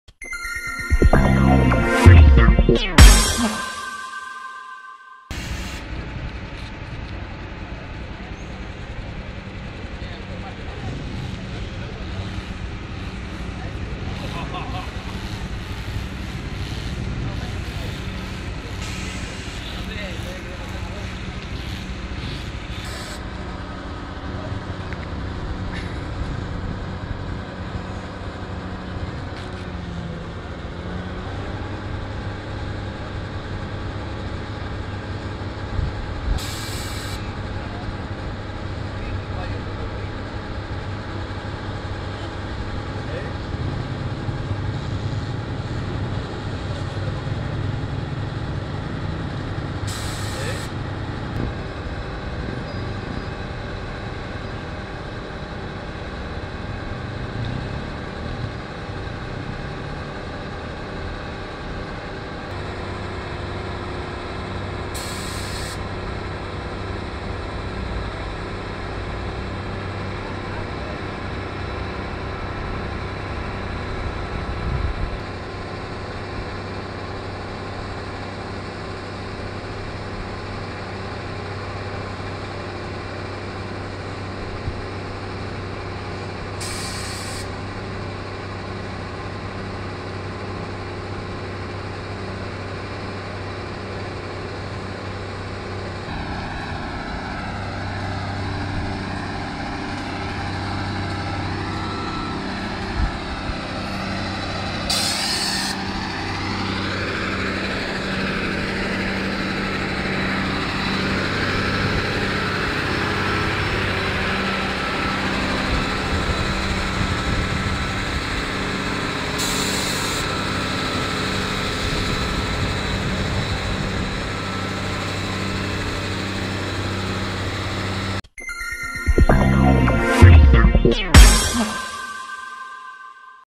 Incendio auto: así trabajaron los bomberos - ANR
Esta mañana un automóvil se incendió en Rosario de Santa Fe al 1900. Los daños fueron totales. Mirá como trabajó la dotación de bomberos voluntarios para extinguir el fuego. 20-02-16. Roca